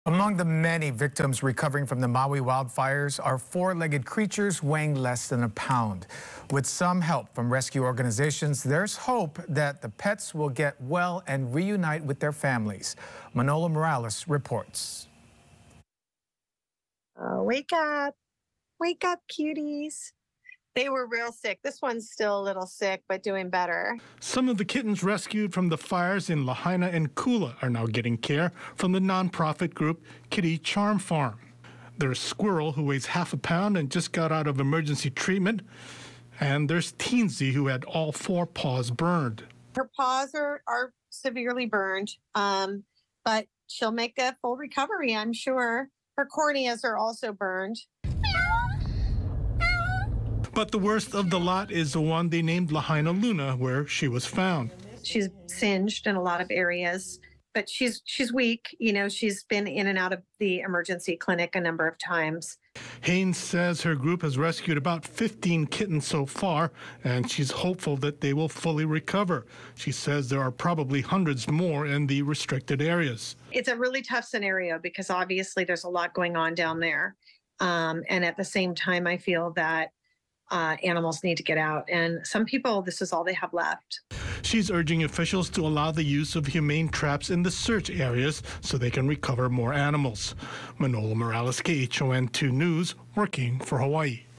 Animal rescue group shares stories of recovery
Among the many victims recovering from the Maui wildfires are four-legged creatures weighing less than a pound.